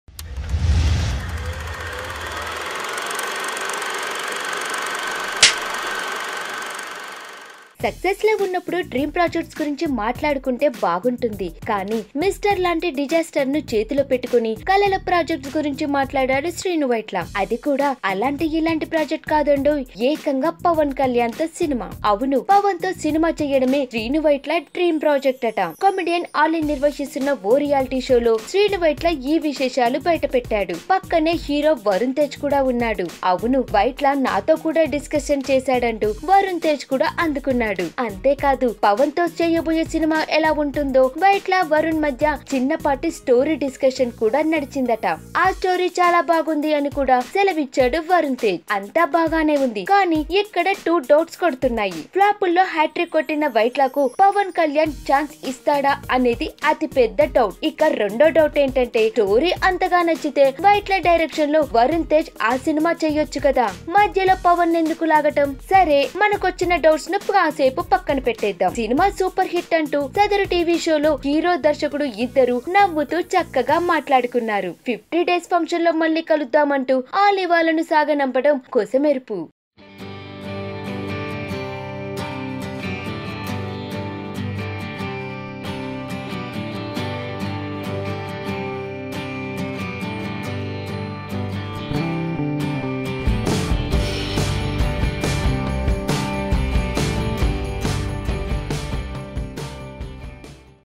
Triple Disaster Director Srinu Vitla Dream Project with Pawan Kalyan - Latest Film News
Watch Triple Disaster Director Srinu Vitla Dream Project with Pawan Kalyan